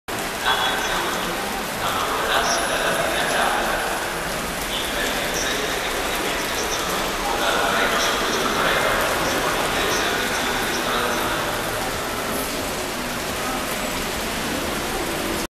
Un poco di pioggia a Porta Susa il 12 aprile 2018
Un poco di pioggia a Torino Porta Susa il 12 aprile 2018